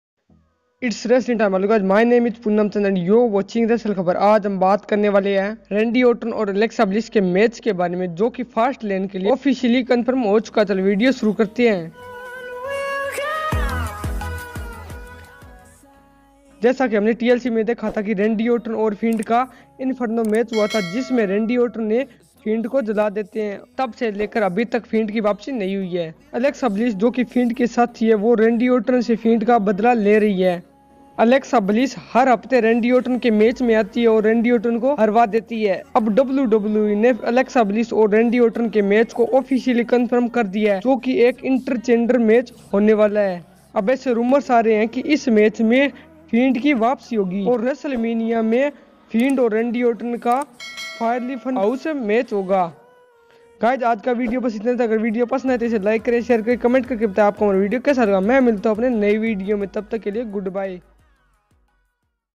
WWE Official Confirmed Randy Orton V/S Alexa Bliss | Fastlane Match Card (Hindi)
WWE Official Confirmed Randy Orton V/S Alexa Bliss | Fastlane Match Card

Video About 👉
 Randy Orton vs Alexa Bliss